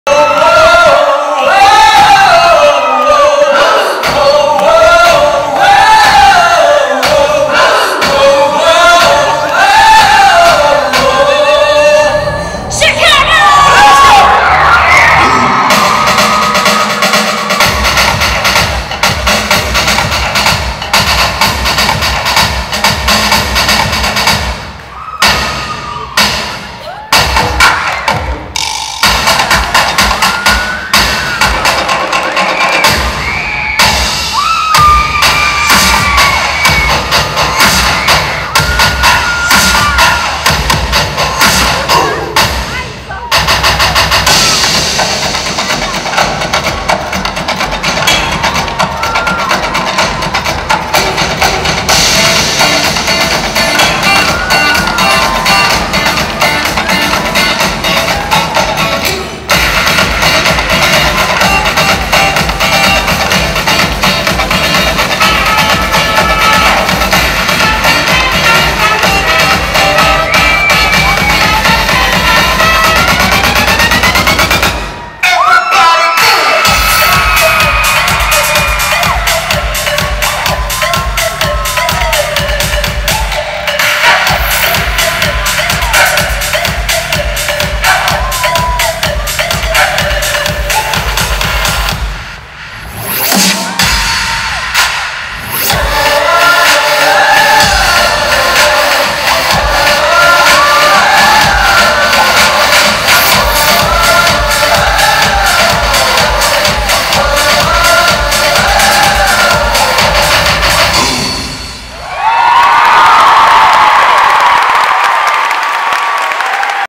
MOVE LIVE ON TOUR (Chicago 25-07-15)
From ARGENTINA to CHICAGO just to cover Julianne and Derek Hough's show.

Chicago Theatre: 25-07-15